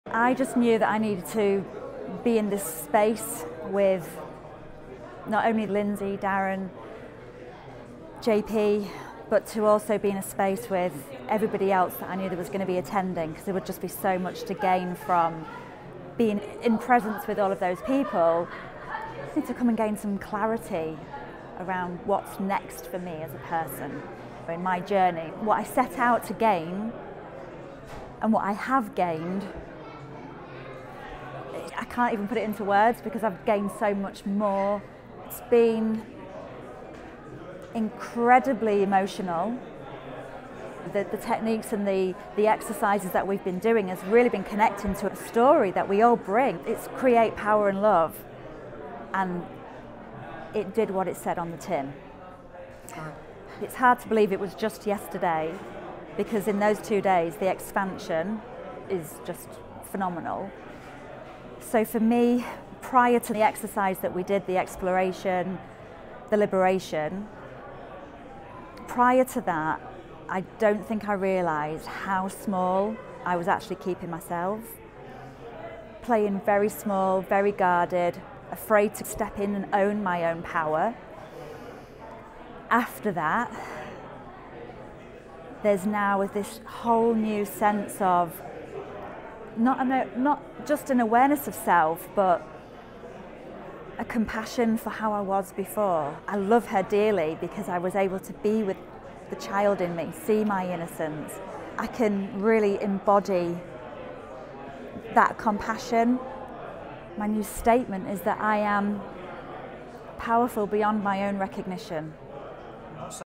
Creating Love & Power | Reflections - Seonaid
Seonaid's Metamorphosis: Embracing Herself to Unlock Infinite Potential.
Join us at Creating Love & Power in Santa Monica, November 9-12th.